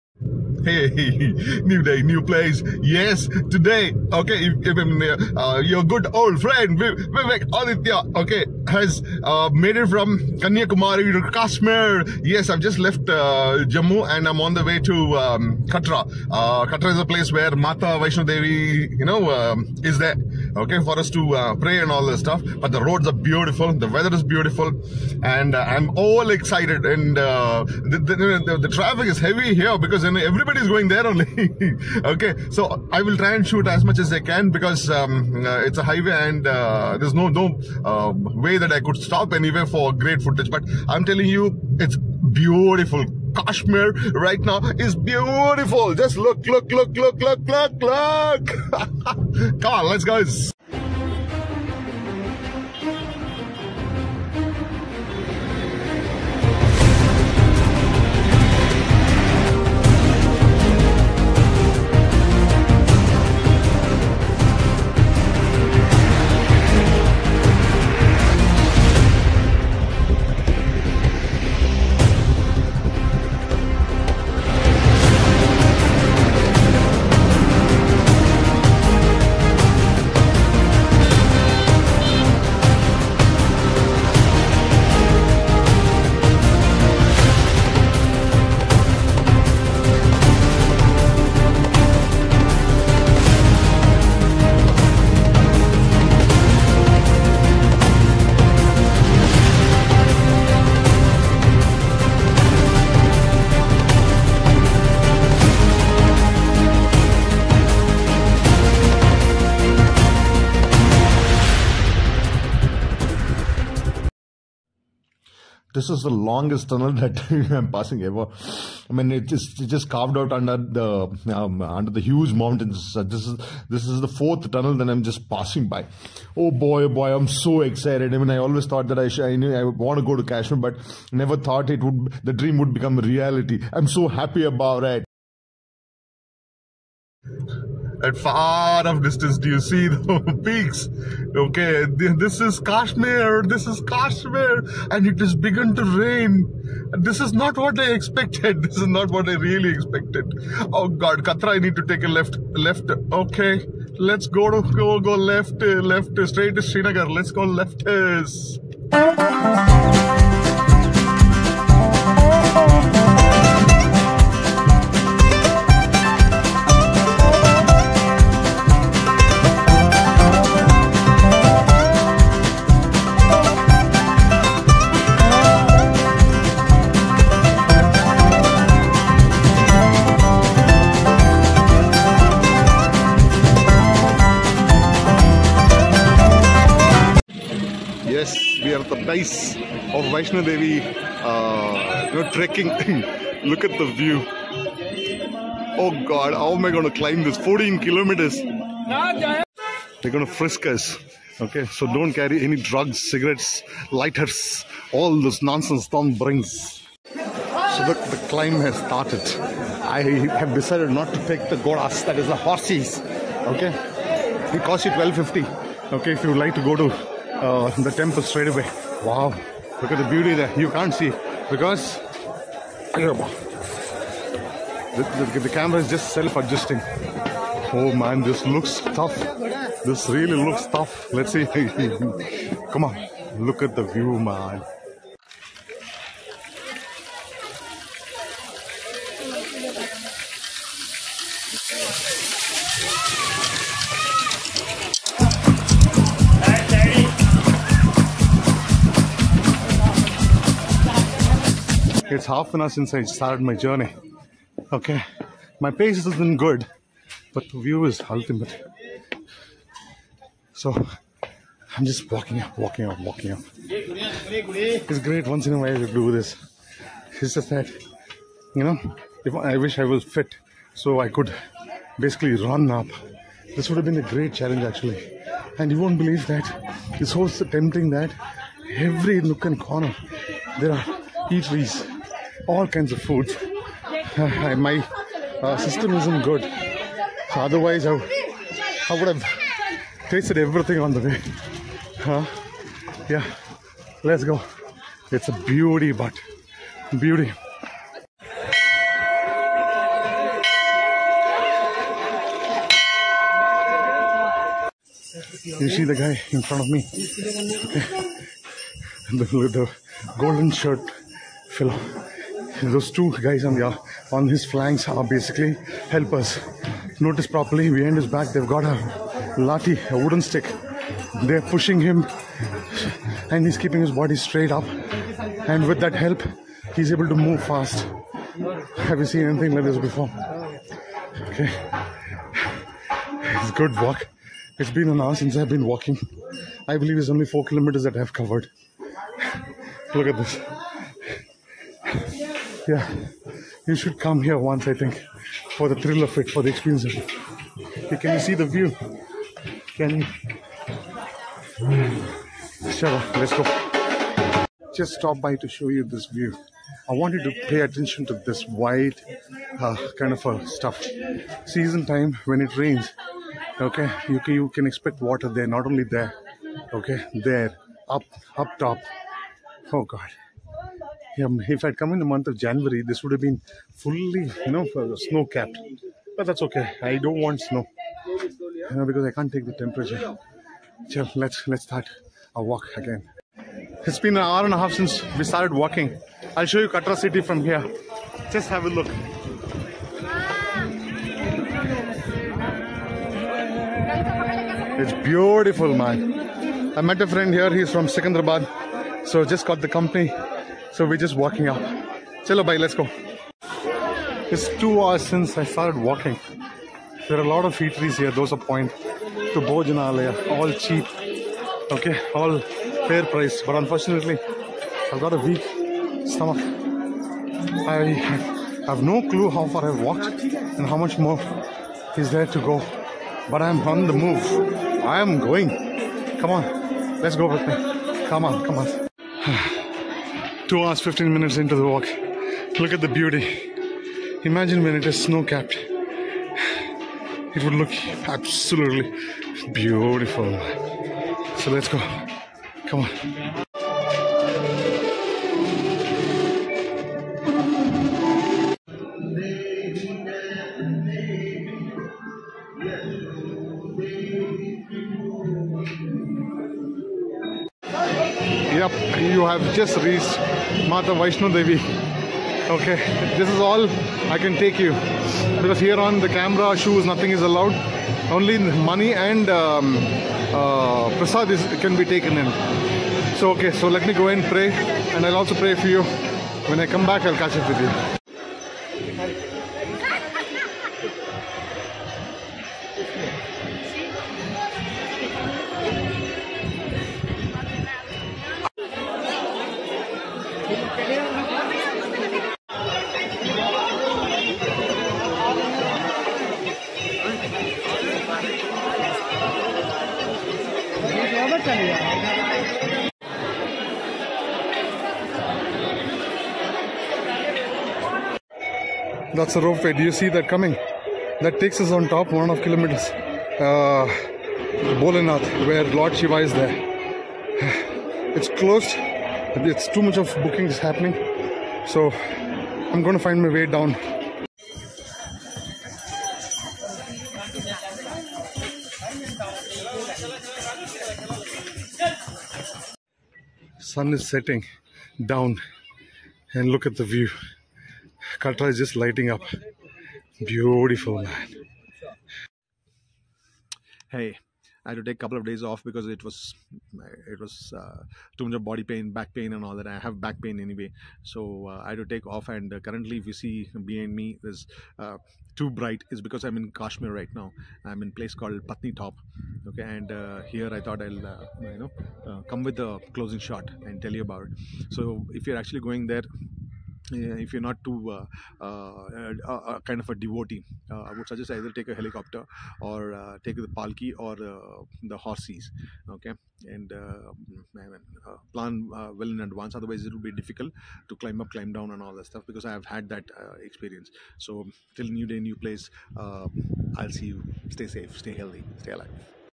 MATA VAISHNO DEVI || HAD TO WALK 28 KM TO PRAY || ALMOST MY BACK BROKE|| THINK MY PRAYERS ANSWERED
mata vaishno devi
Trek to mata vaishno Devi
places to visit in katra
pilgrimage